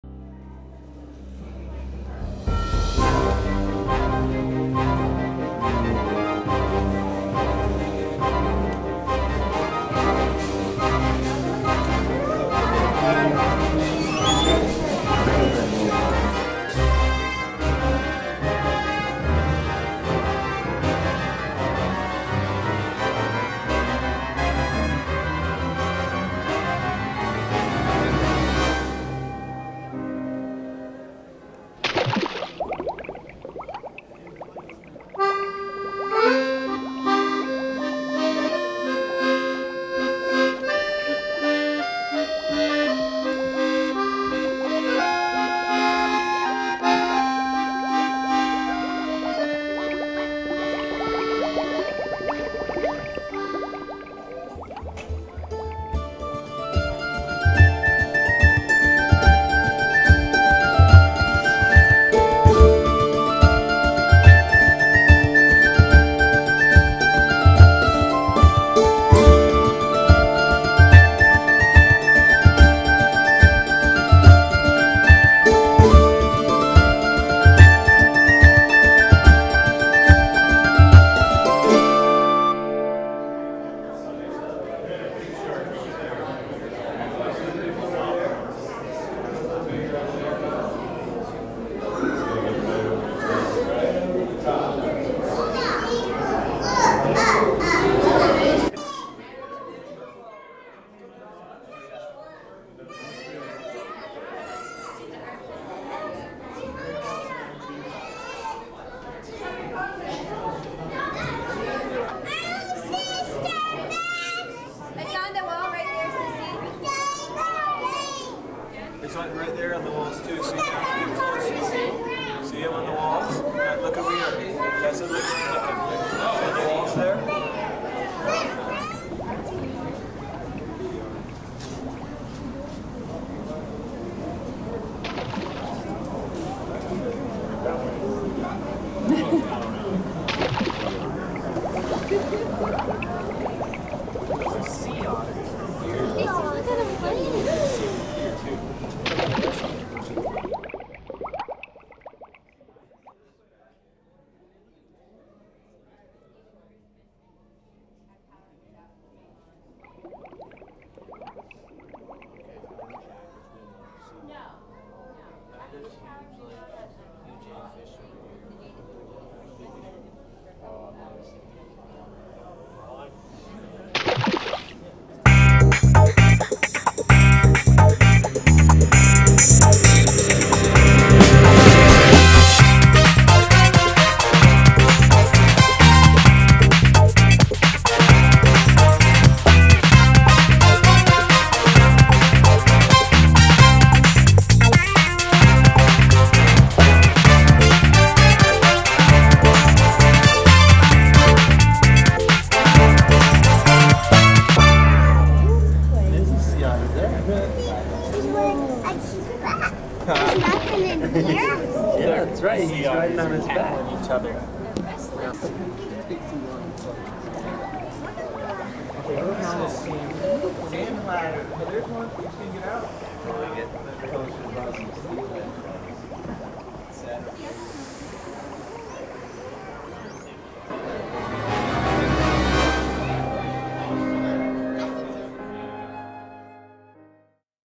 Puget Sound Fish and Playful Sea Otters
Scenes shot at the Seattle Aquarium in January, 2007.

When it comes to playful swimmers at the Aquarium, everybody loves one or the otter .